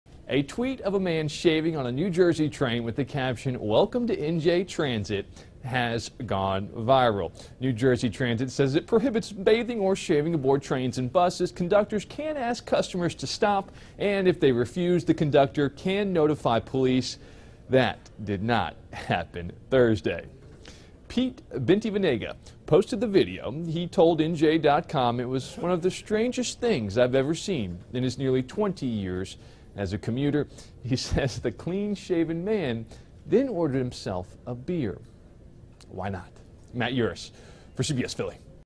Careless Whiskers: Train Commuter Caught On Video Shaving
A man caught on video lathering up and giving himself a shave while riding on a New Jersey train was violating the agency's rules. Matt Yurus reports.